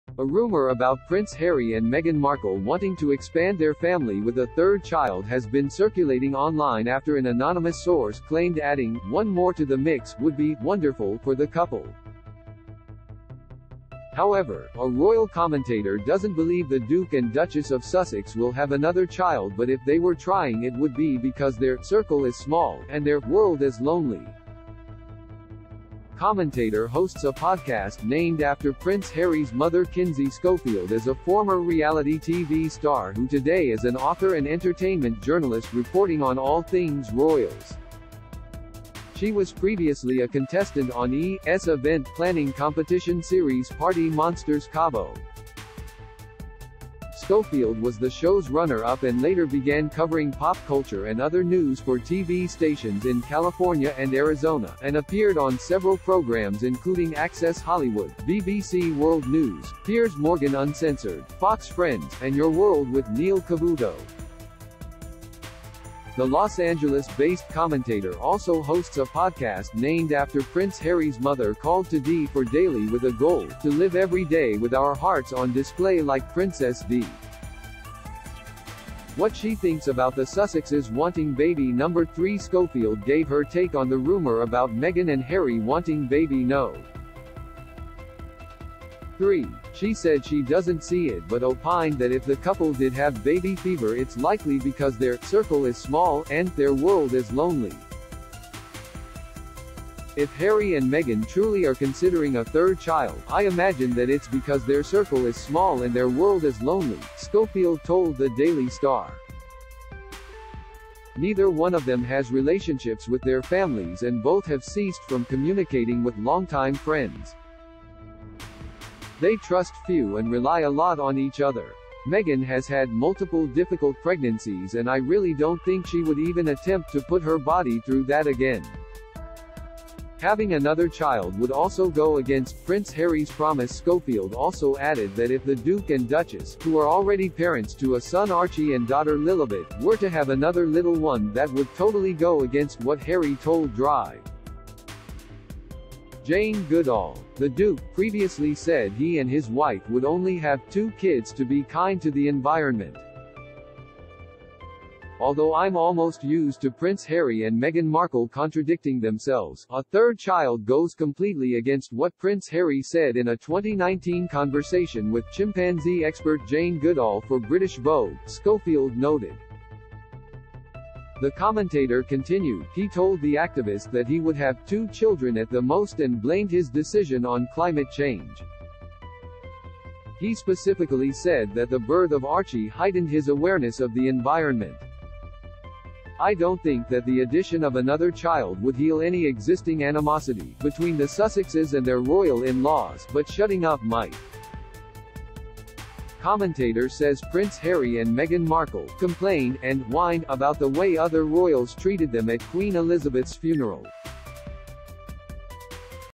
Royal Commentator Says Prince Harry and Meghan Markle 'Trust Very Few People' and 'Their World Is Lo
A rumor about Prince Harry and Meghan Markle wanting to expand their family with a third child has been circulating online after an anonymous source claimed adding “one more to the mix” would be “wonderful” for the couple.   However, a royal commentator doesn’t believe the Duke and Duchess of Sussex will have another child but if they were trying it would be because their “circle is small” and their “world is lonely.”  Commentator hosts a podcast named after Prince Harry’s mother   Kinsey Schof